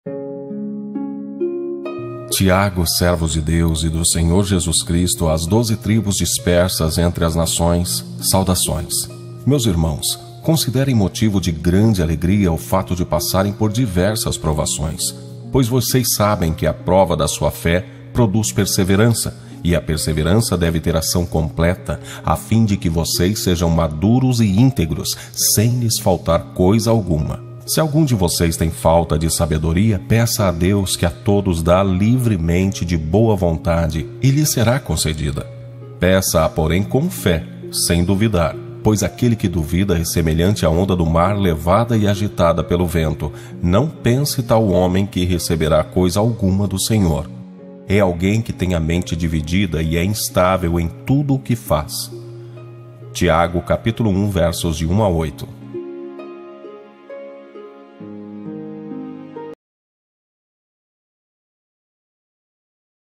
60 SEGUNDOS DE BÍBLIA Dia 07 de Outubro 2019
Tens um minuto pra Deus? Então ouça 60 segundos de Bíblia pura. Tradução NVI - Nova versão internacional. Narração: Ney Santos

FUNDO MUSICAL DE CÍCERO EUCLIDES